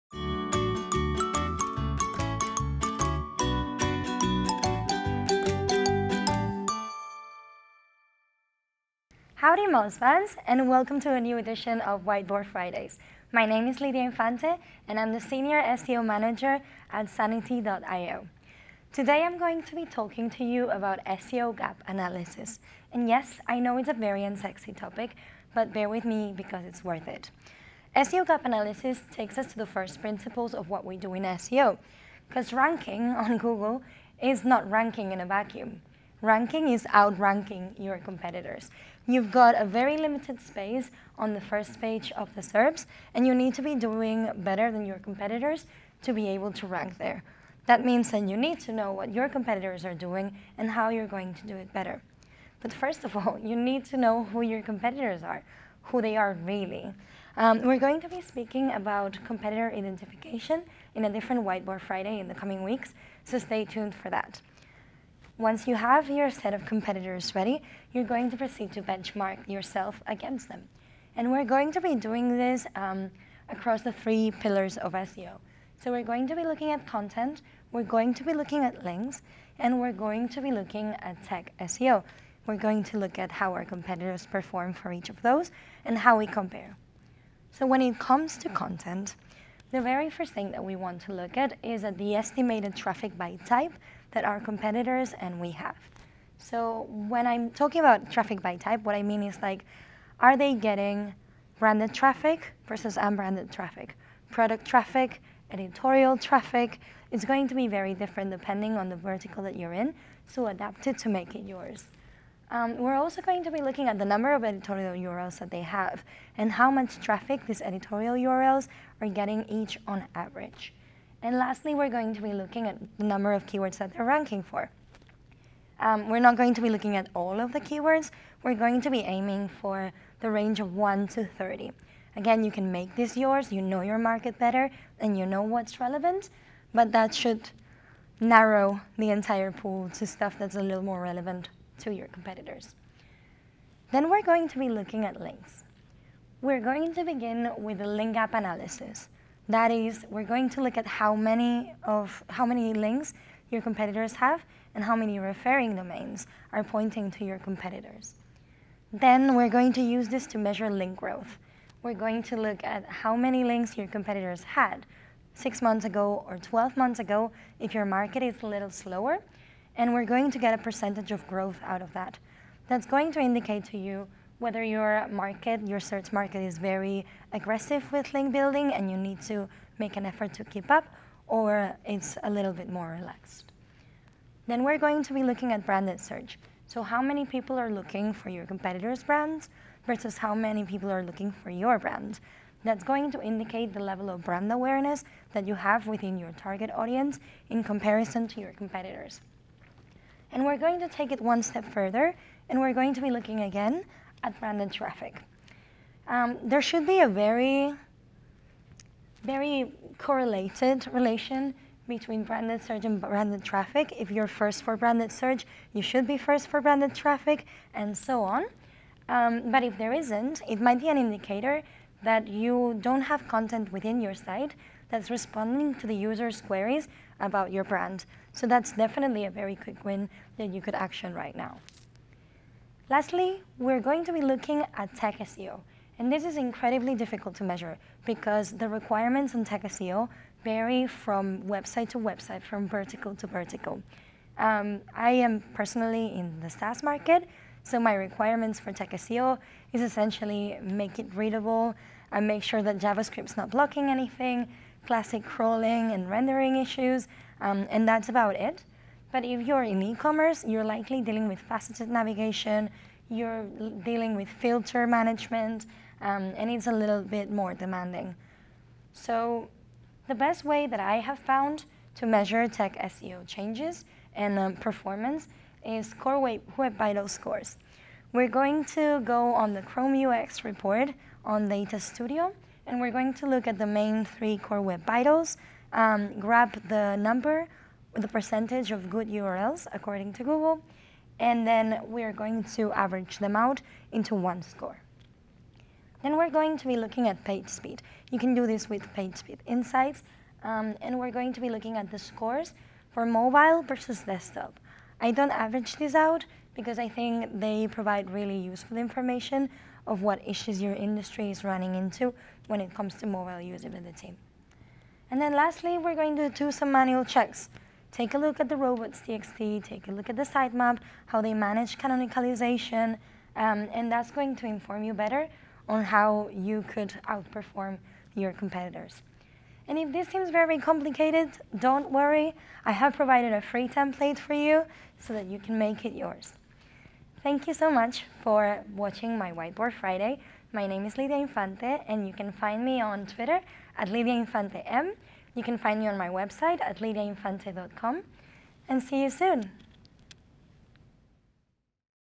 SEO Gap Analysis — Whiteboard Friday
Ranking on Google is not ranking in a vacuum. Ranking is outranking your competitors. When you've got very limited space on the first page of the SERPs, you need to be doing better than your competitors. One way to achieve this? Competitive gap analysis.